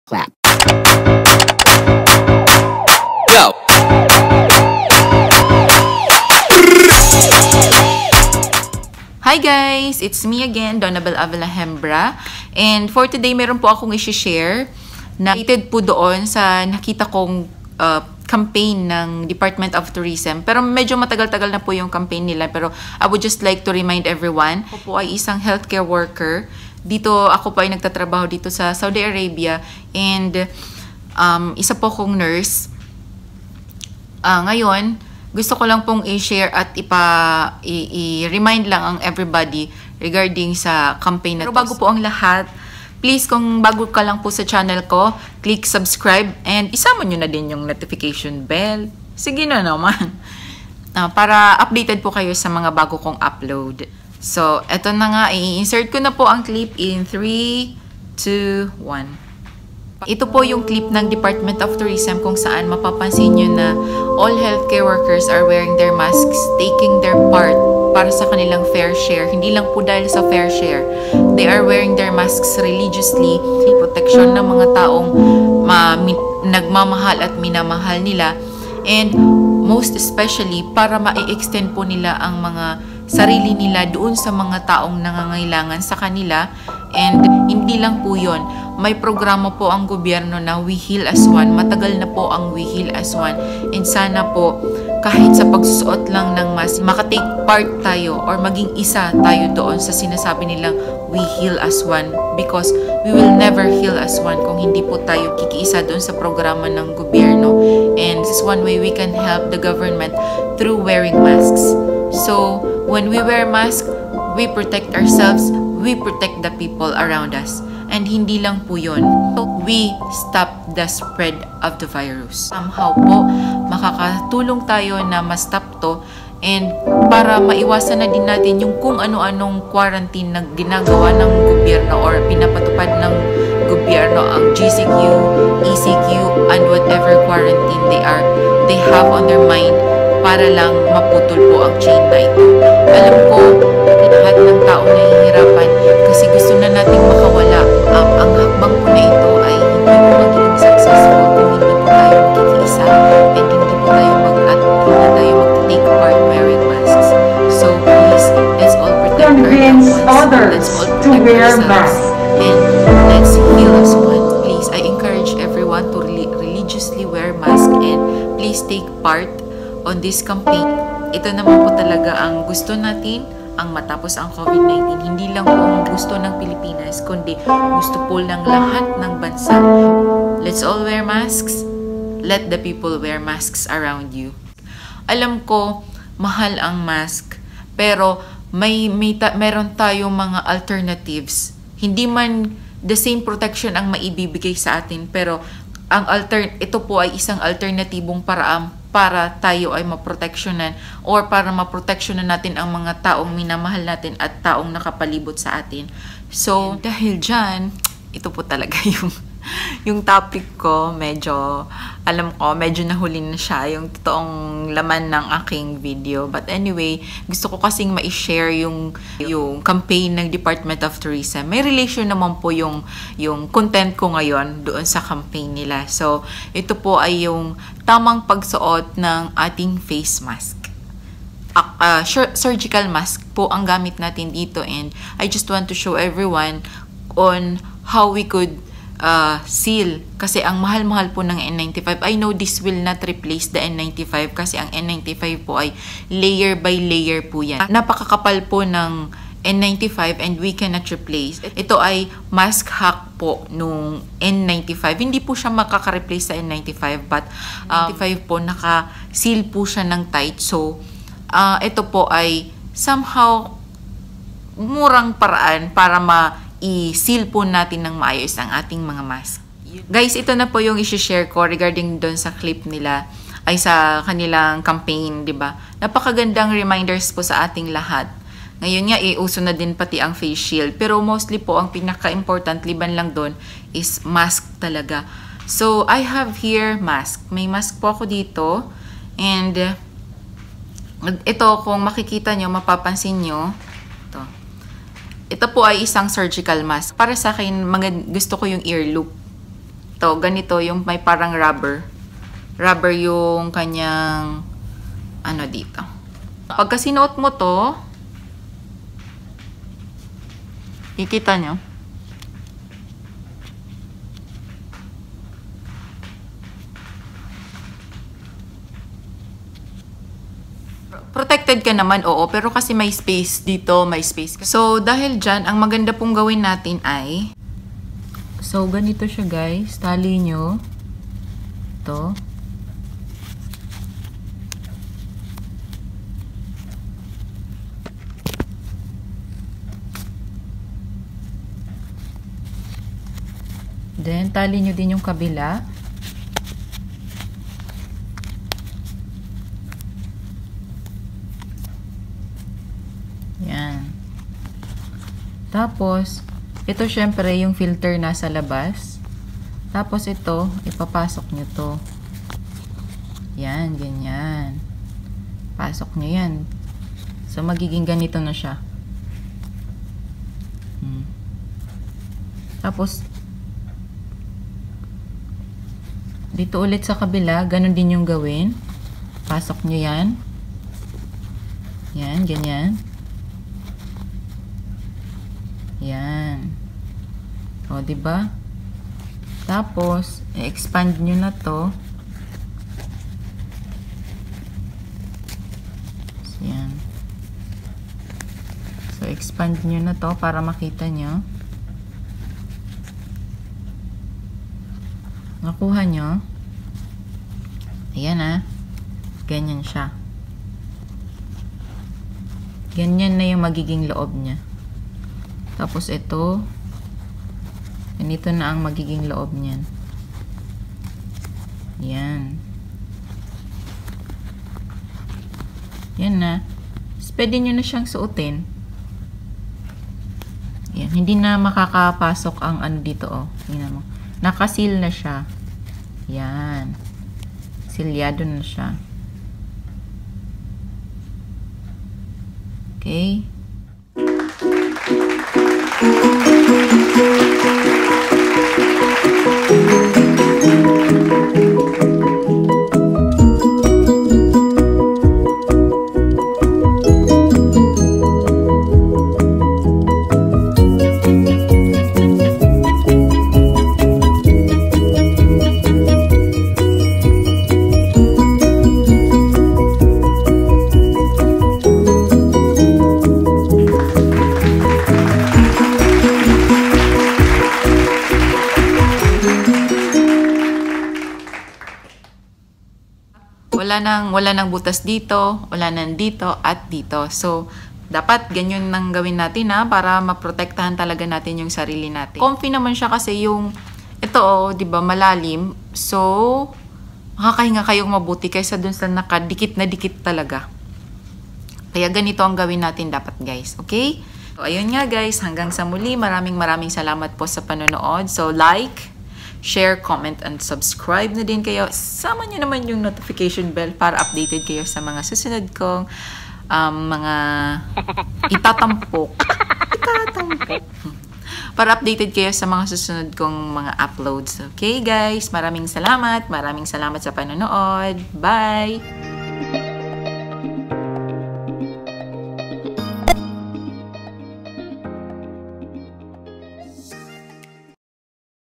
ITS ONLY A MASK! | DOT CAMPAIGN I KSA NURSE
ITS ONLY A MASK
DEPARTMENT OF TOURISM CAMPAIGN BY @CONG TOURS

dahil sa pandemya mahirap talaga ang magsawalang action. sa simpling pagsusuot ng mask maaari na tayong makatulong na mabawasan ang spread ng virus na ito.
panoorin ang aking video para malaman ang tamang pagsuot ng mask.

LETS stop the spread of the virus
LETS ALL WEAR MASK